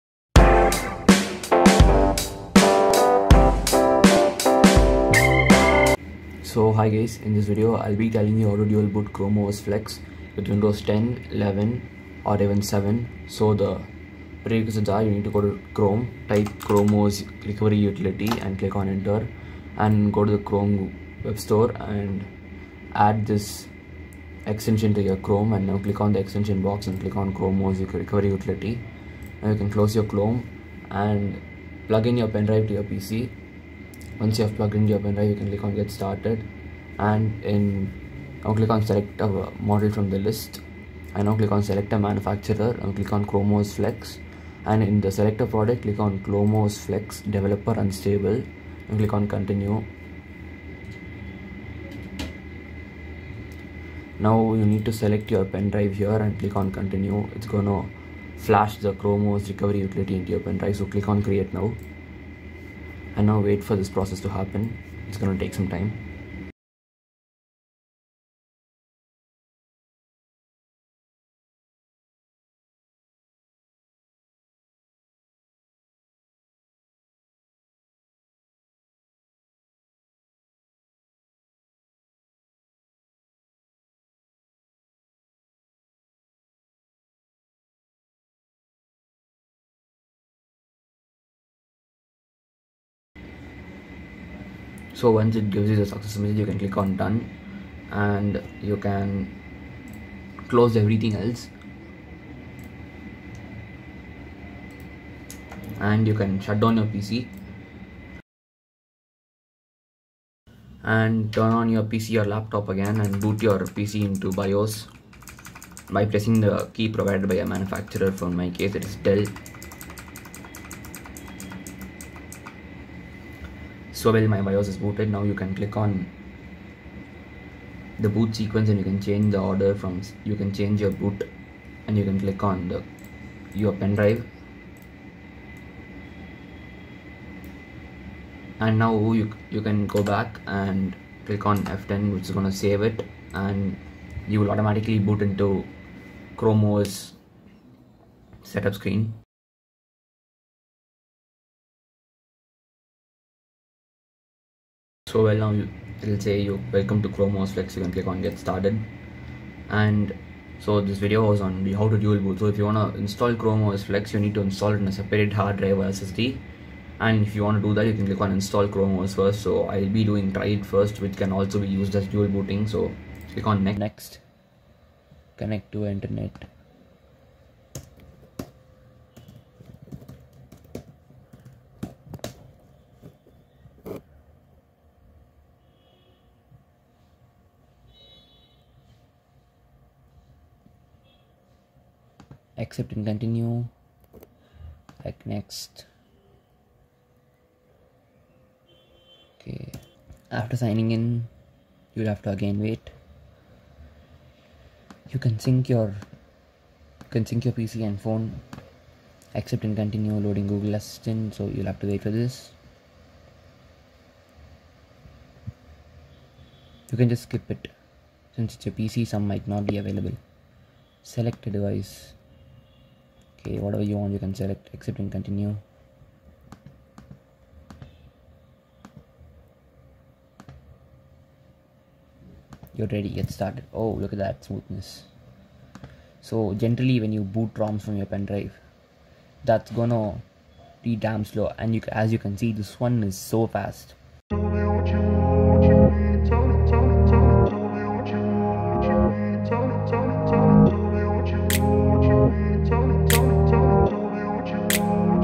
Chrome OS Flex: Download & Install on PC | Dual Boot | No Data Loss | Play Store | 2022
Hi Guys This Video is ON how to Install / Dual Boot Chrome os Flex

Requirments -
1. Coumputer
2. Pendrive ( 8GB )
3. Brain